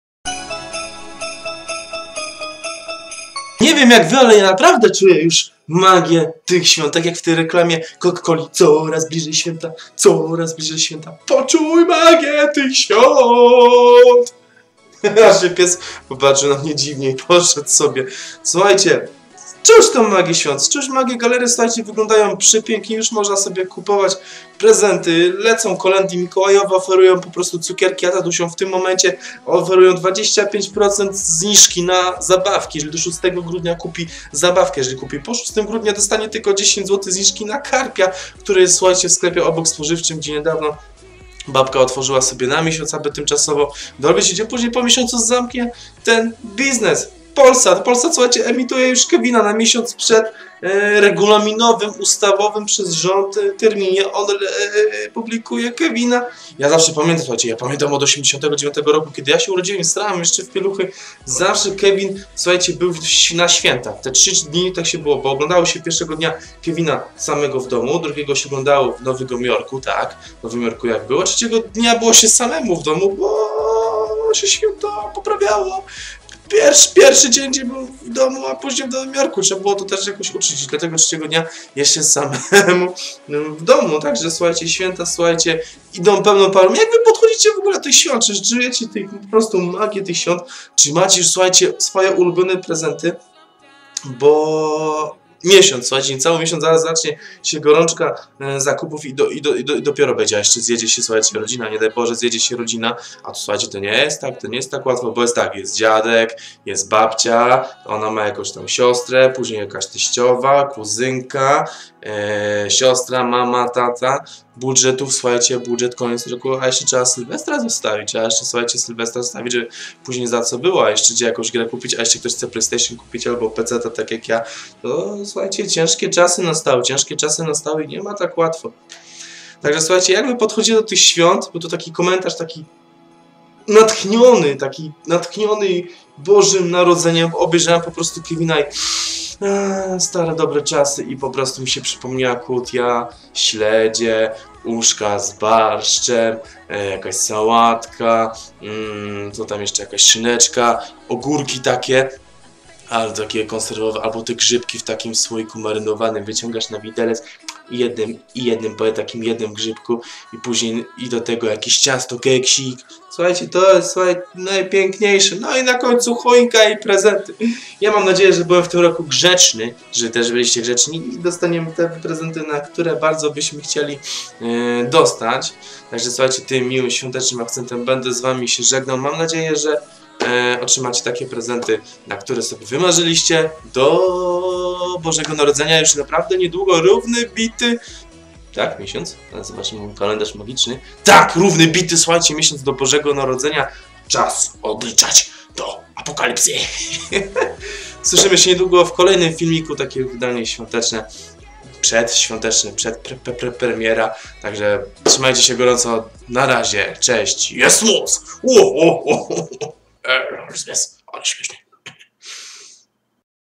Kevin sam w domu,prezenty,karp i kochana familia. Nadchodzą Święta
Do świąt pozostał jedynie miesiąc.Czas podjąć rękawicę i stanąć do pojedynku z gorączką świąteczną ho ho ho JEST MOC!